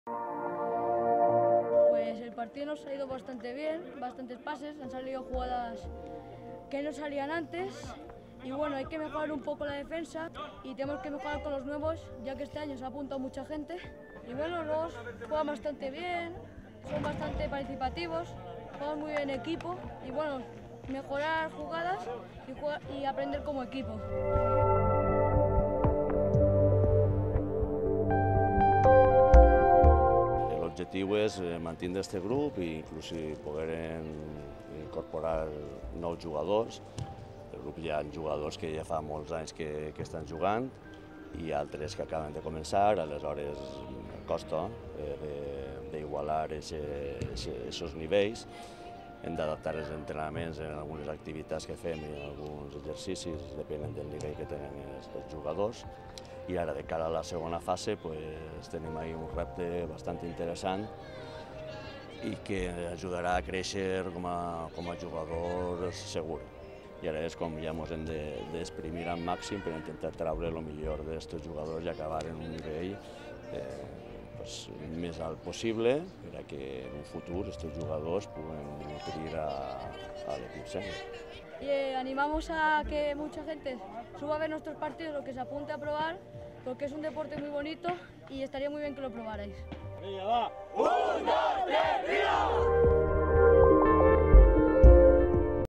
Recuerdos del S16 / Antes del COVID19
Entrevistas inéditas de Adrián, jugador S16, y Kiko Bou, entrenador S16, antes de la crisis del coronavirus. Ánimo!!! Volverán los entrenamientos!!! Pero ahora